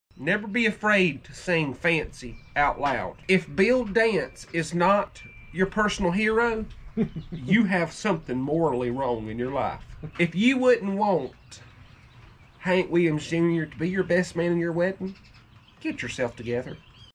All American Adventures. Hunter's Rules
Hunter shares his Three Rules.